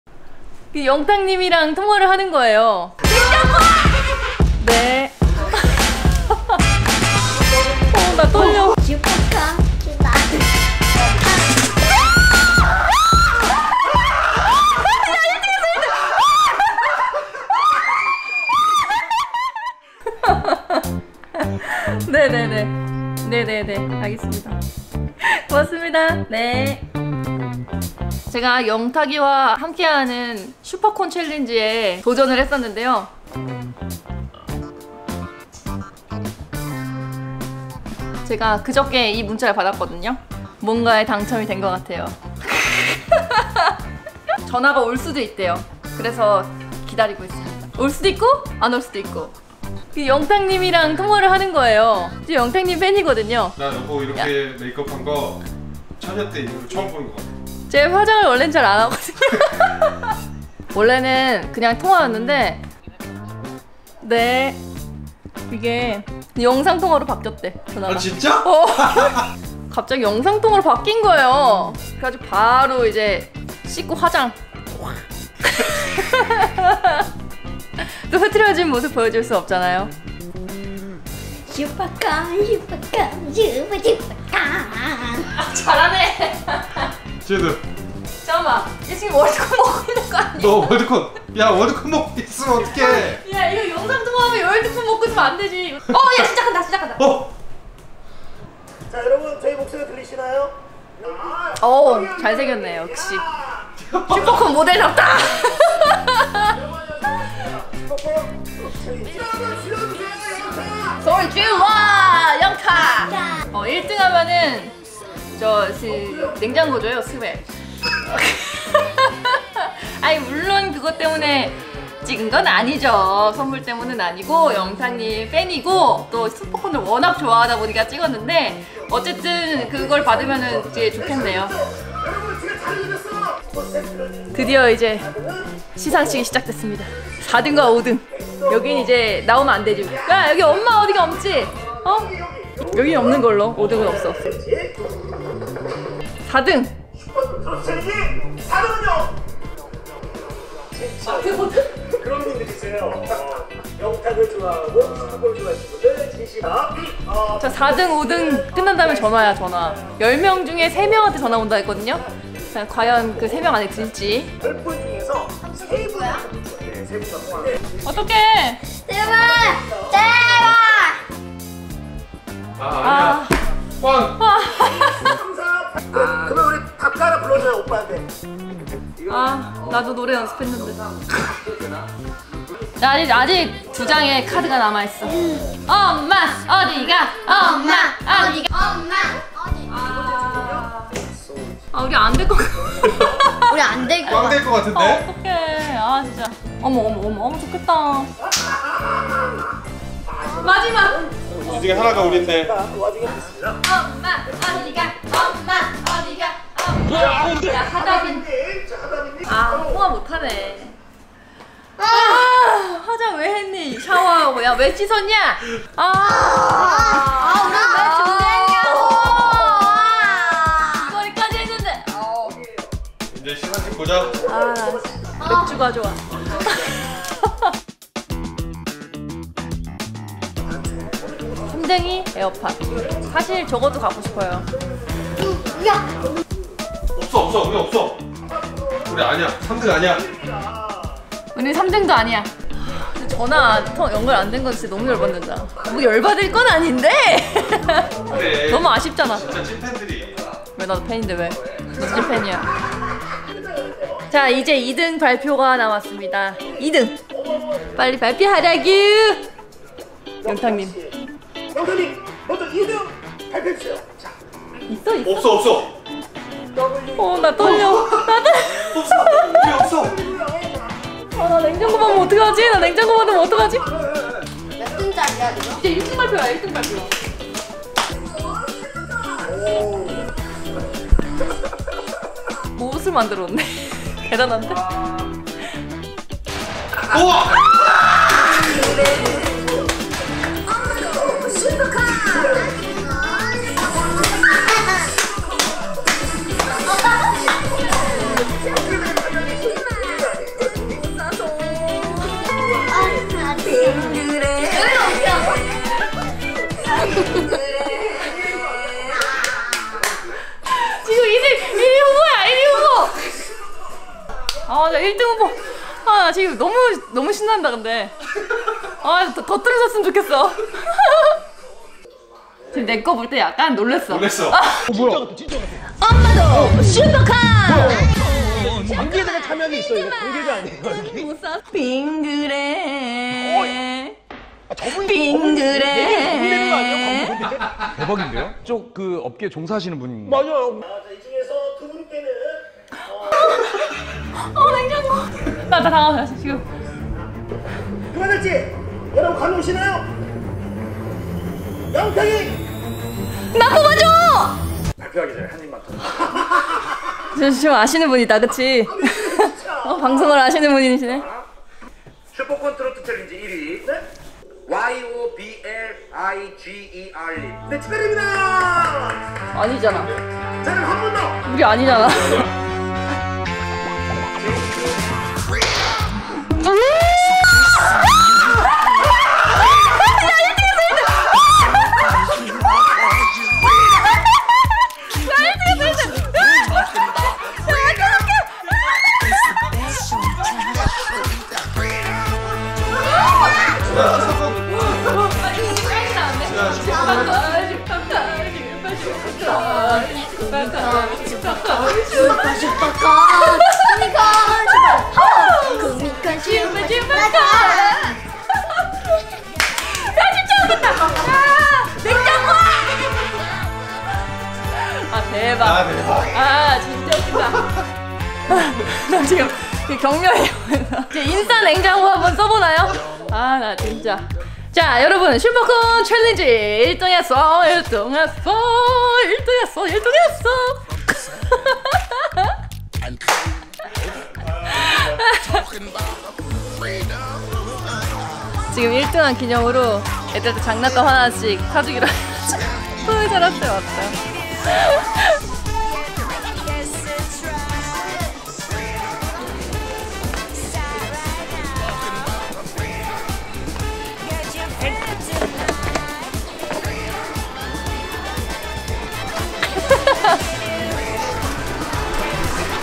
[1등하는 브이로그] 슈퍼콘 챌린지1등!!! 영탁님이랑 영상통화
얼마전에 도전했었던 슈퍼콘 트로트 챌린지에서 1등의 영광을 얻었습니다

소리 질러~!!! 예~~~

1등 선물로 냉장고 받고  영상통화 기회도 얻었습니다.  통화는...

항상 행복한 일만 가득한 우리집  ^^

 축하해주시고 응원해주세요 헤헹